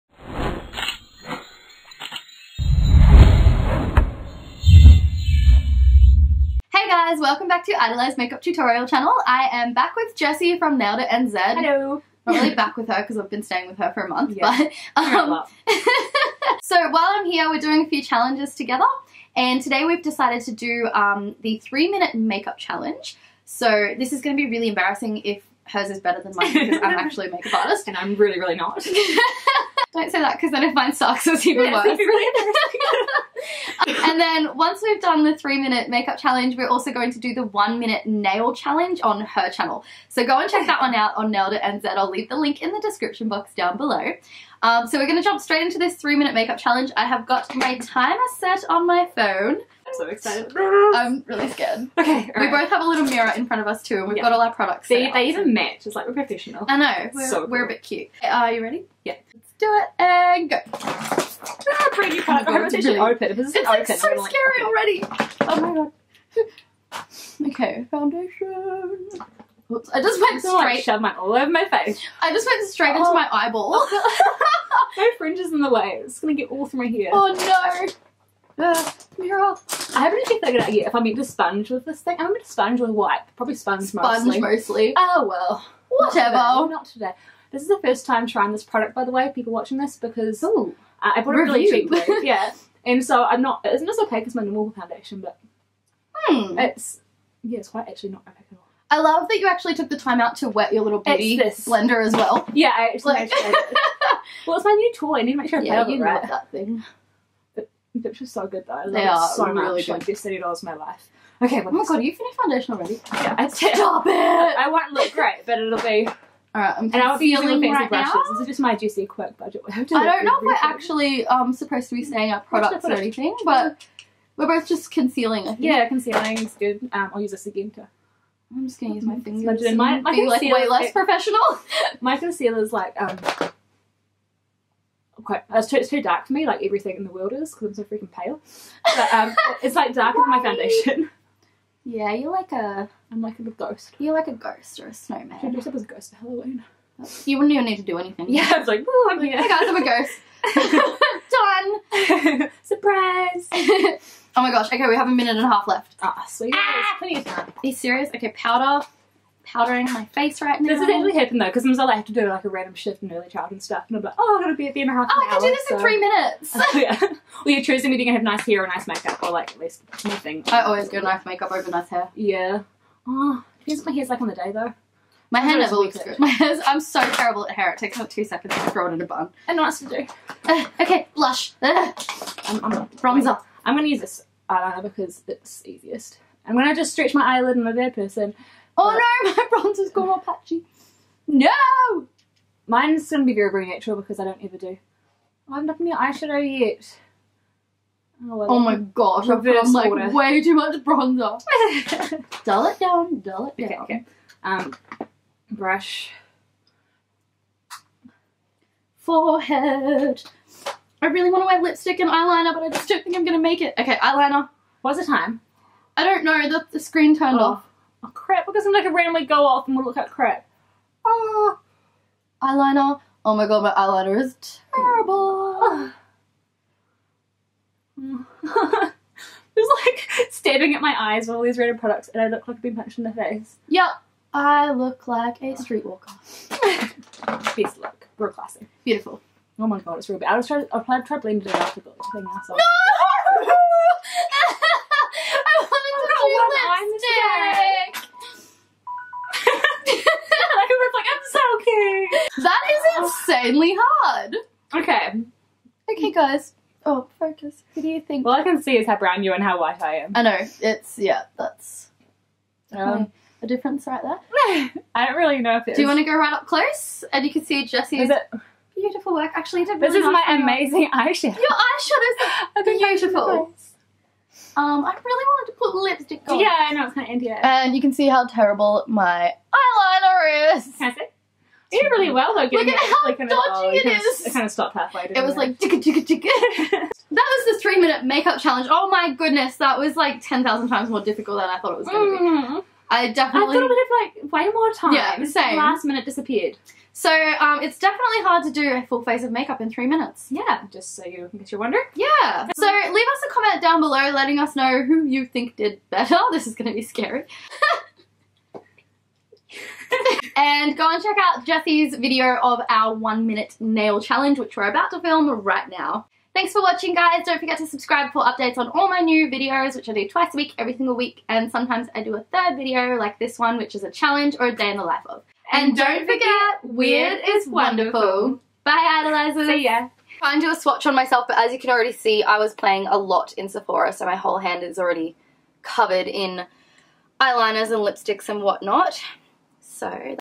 EyedolizeMakeup Travels: 3 Minute Makeup Challenge with Nailed It NZ
Hey Eyedolizers!!! Today I teamed up with Jessie from Nailed It NZ to try and attempt the 3 minute makeup challenge - check out the ridiculous results in this video! :) xx

Also watch: "The PERFECT Job Interview or Meeting Makeup by EyedolizeMakeup"